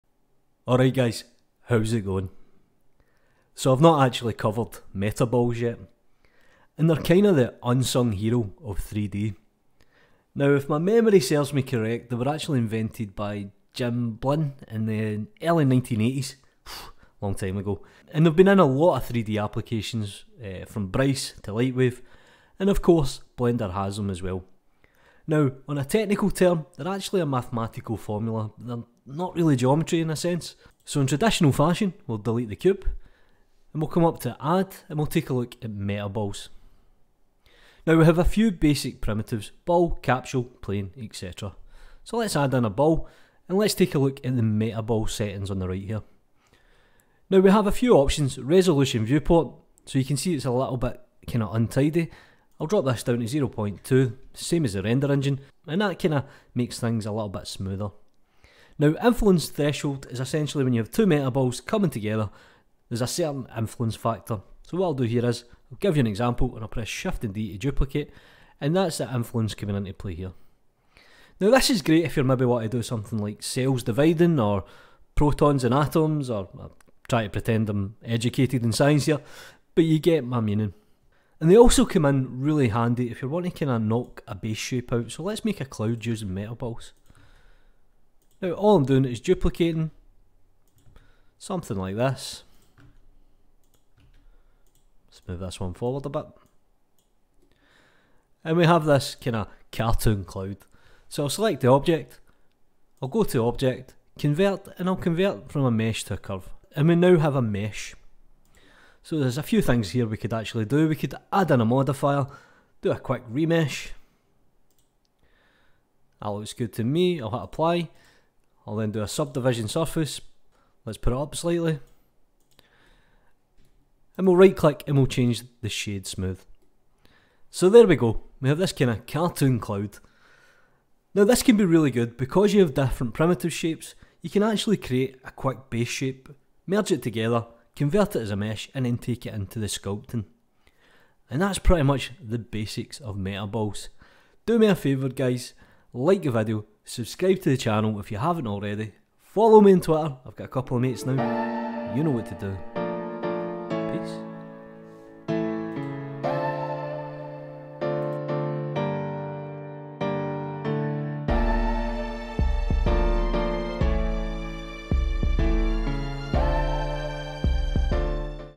Blender 2.8 Tutorial: Metaball explained ... the unsung heroes of 3D
Metaballs have been in 3D from the dawn of time but that doesn't mean they dont come in useful... quickly make a cartoon cloud using metaballs.